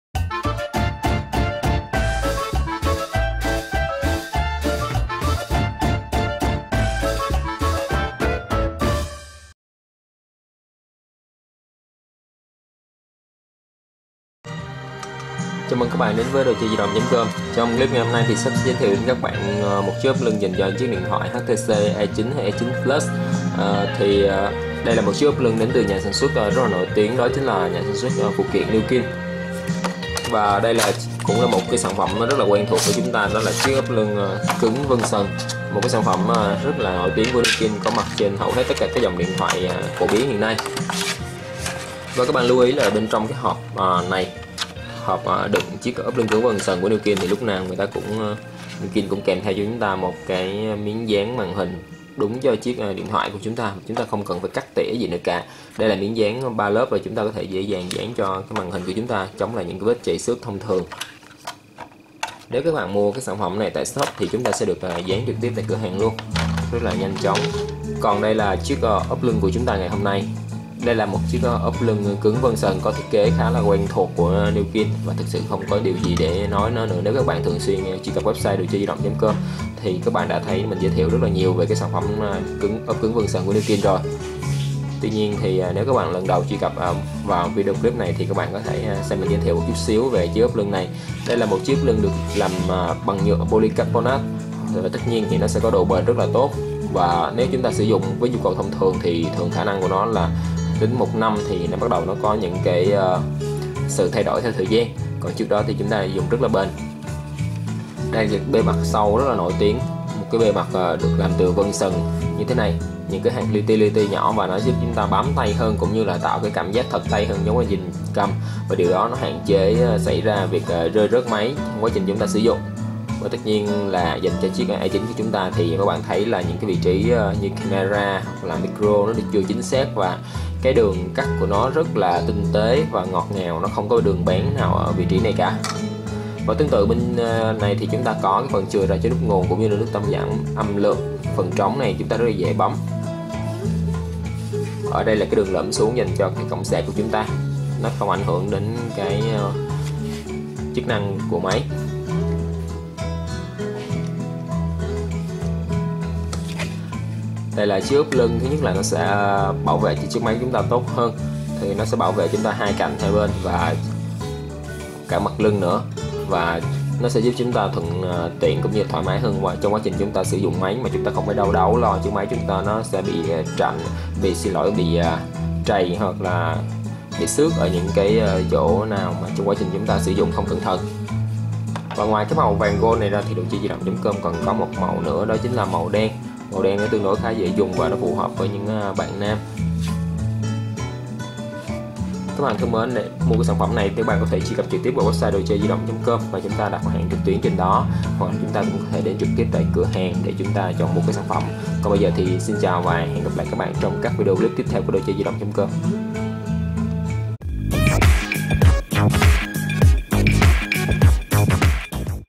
Trên tay Ốp lưng HTC One E9 Nillkin vân sần - Đồ Chơi Di Động .com
Ốp lưng HTC One E9 Nillkin vân sần
👉 Mã sản phẩm: DCDD3564
💰 Giá chỉ có 120k
🏠 Địa chỉ: 206 Nguyễn Thượng Hiền, P.5, Phú Nhuận, TP.HCM